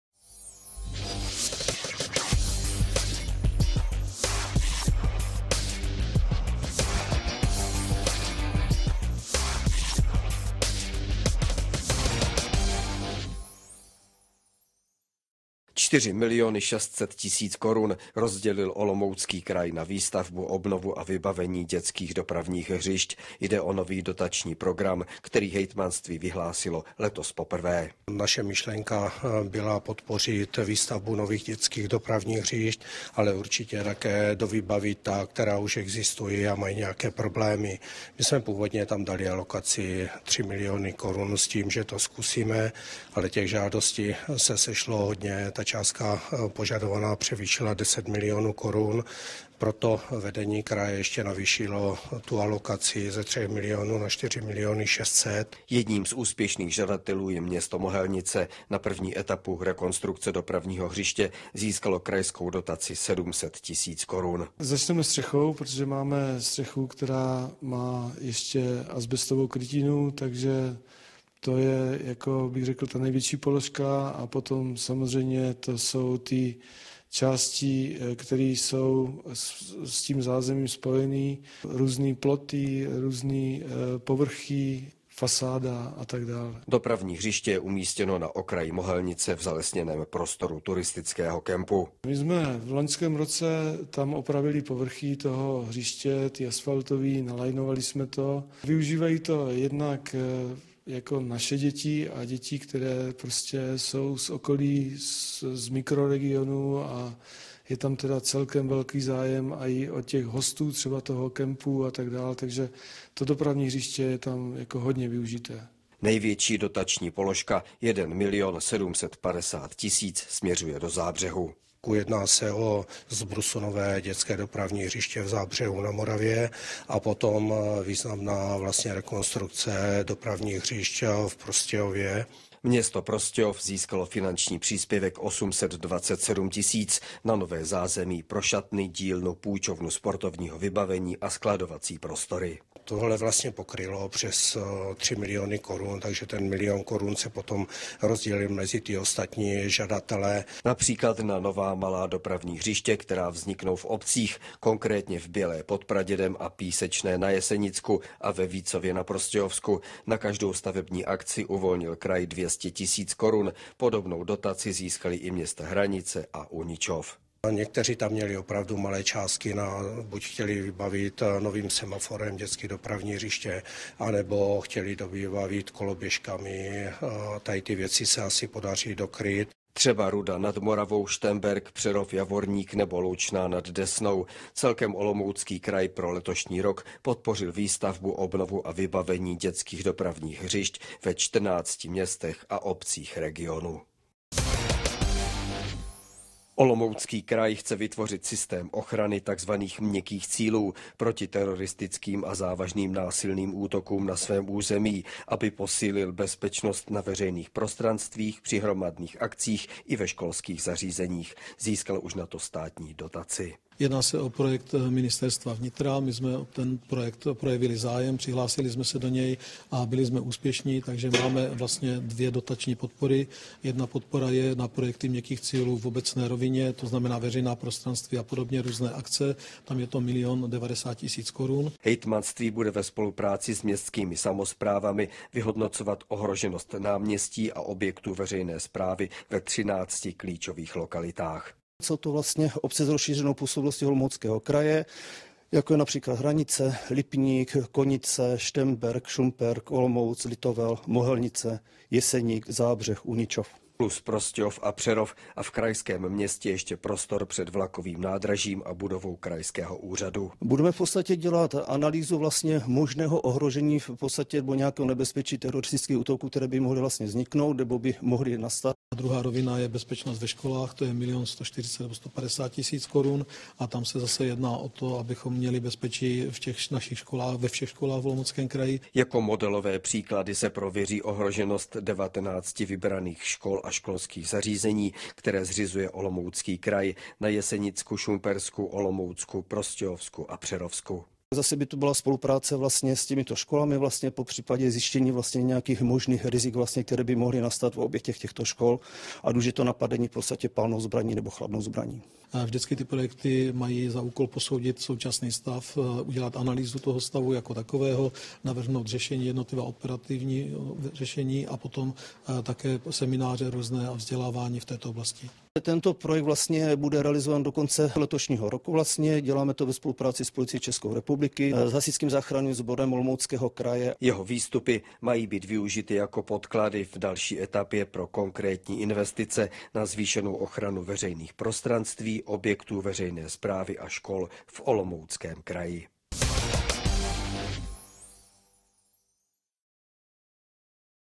NÁŠ KRAJ - 20. TÝDEN 2019
📺 Letos jsme poprvé spustili dotační program na výstavbu, obnovu a dovybavení dětských dopravních hřišť. Úspěšní žadatelé si rozdělí přes čtyři a půl miliónu korun.

📺 Získali jsme státní dotaci na posílení bezpečnosti na veřejném prostranství, při hromadných akcích a ve školách.